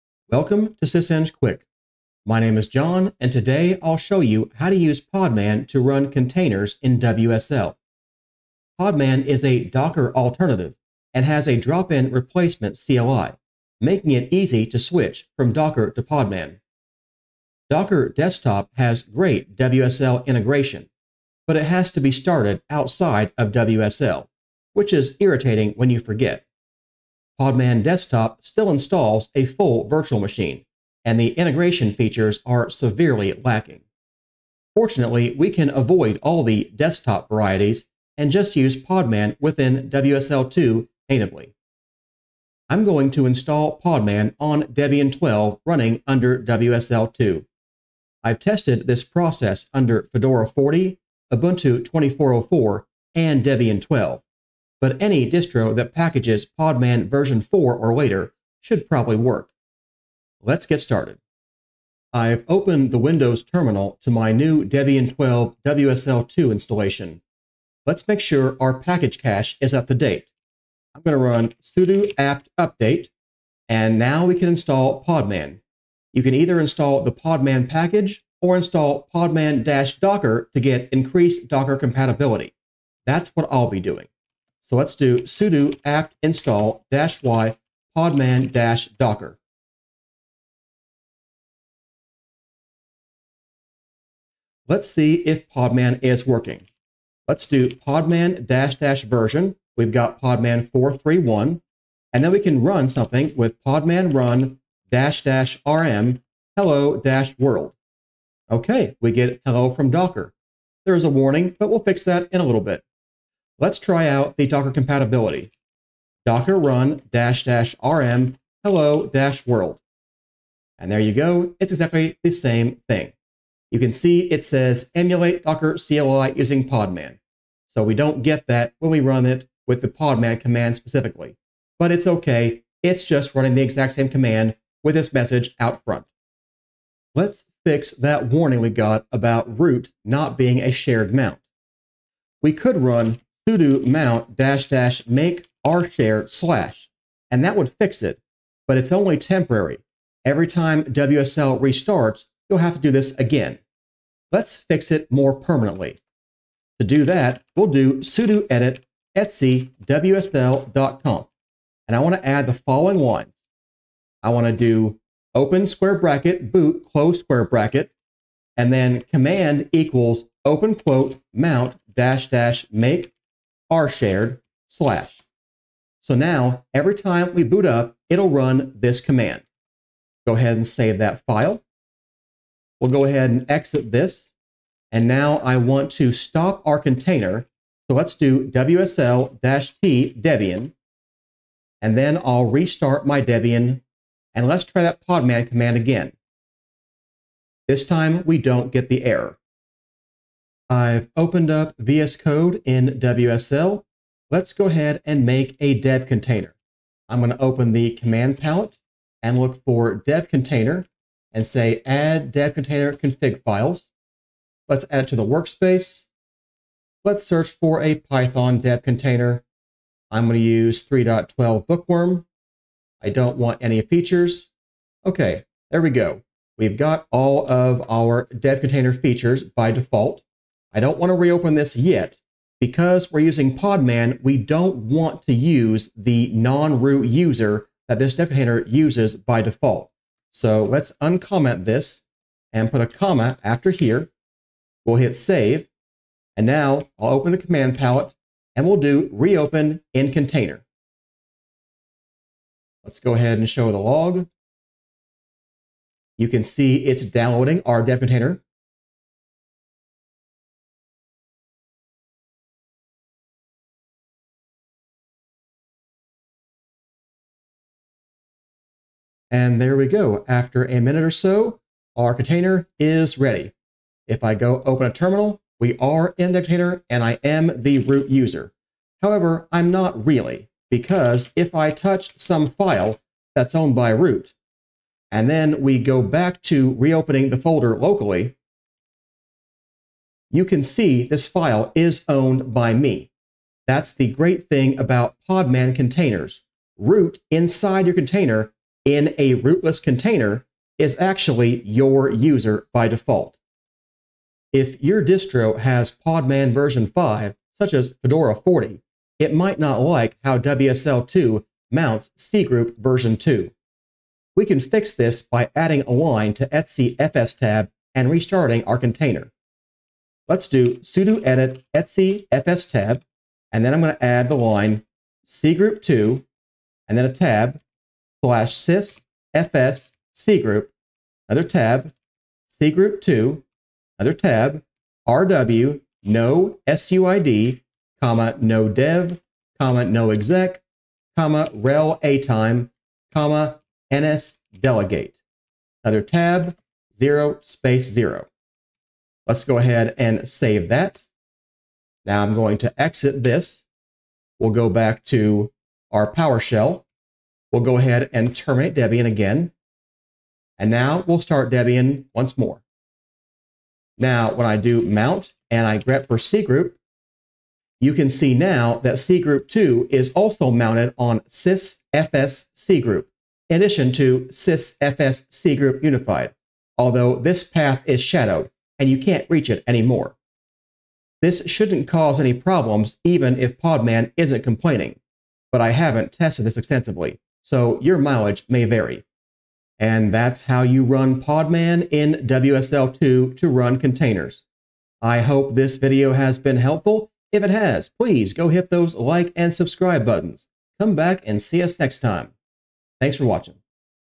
Podman Containers in WSL
Short tutorial on using podman to run containers in WSL instead of docker.